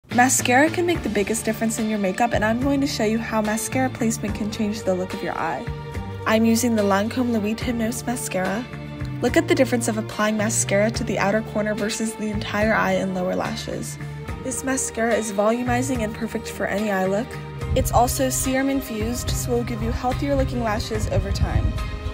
Geena Hunt gets instant volume with Le8Hypnose Mascara! Our first serum-infused mascara.
Get instant volume with Lancôme's serum-infused volumizing mascara, Le 8 Hypnôse. This mascara is formulated with 91% black balm, 8 revitalizing amino acids and shea butter. Pro tip: Coat lashes with Cils Booster XL Lash Enhancing Primer for extra drama.